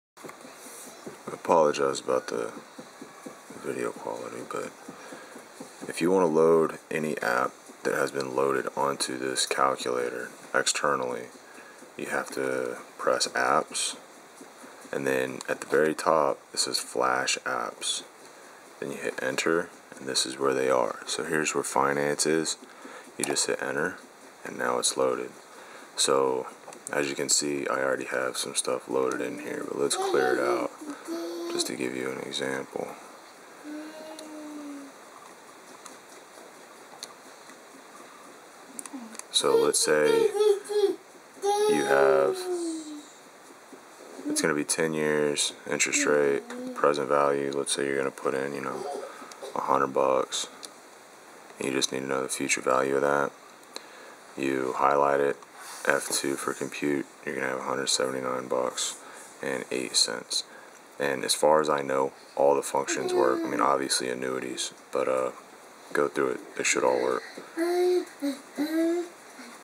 How to Open Finance App CellSheet, etc. on the TI-
The first 2 builds of the TI-89 are not the Titanium.  The additional apps you load, such as cell sheet, finance, etc. must be loaded via the Flash Apps.  So you press APPS then FlashApps then Select the App you want to load